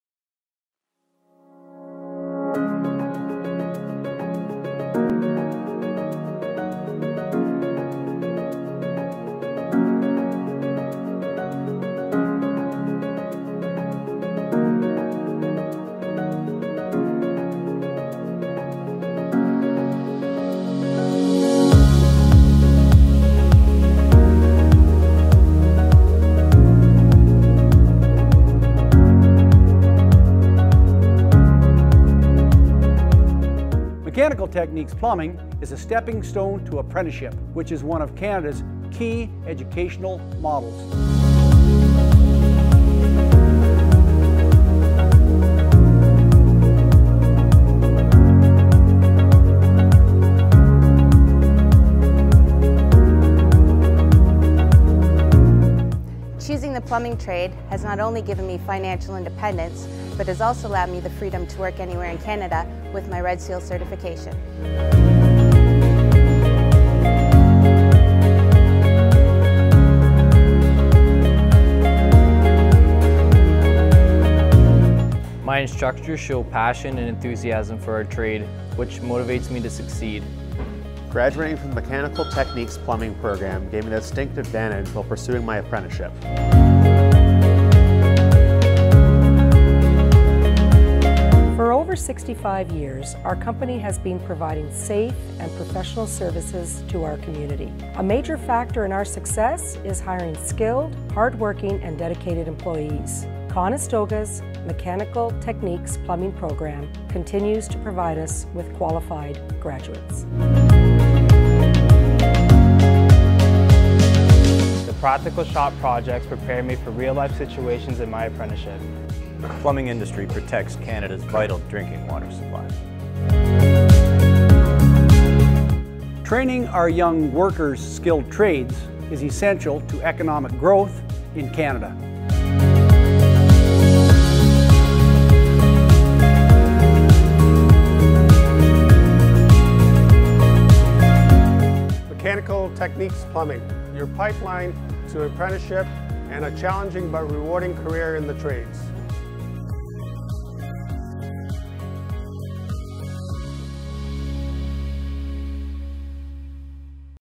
Mechanical Techniques - Plumbing (1245)
This program provides you with entry level practical skills needed to enter the plumbing trade, safe and practical application of plumbing techniques and methods relating to residential and commercial installation and repair. Skills training will include identification of fittings and plumbing related materials and plumbing joinery in accordance with industry safety standards and the Ontario Plumbing Code.